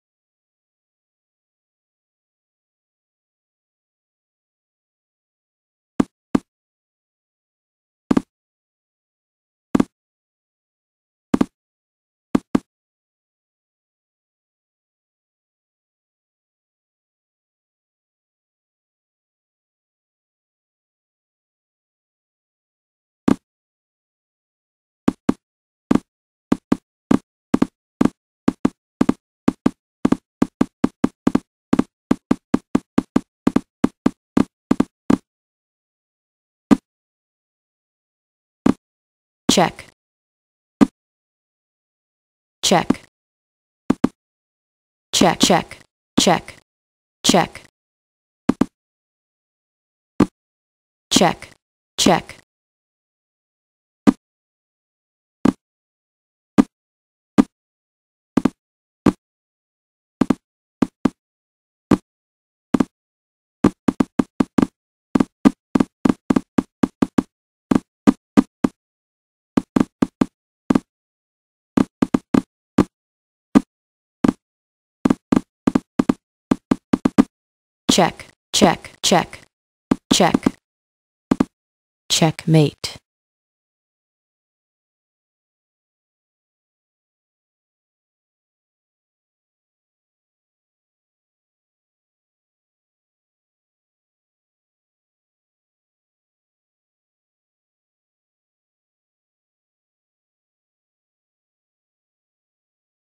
♚ GM Jeffery Xiong vs Computer StrongBach 🔥 Bullet Chess on ICC April 19, 2017
HD
GM Jeffery Xiong vs Computer StrongBach

Chess Channel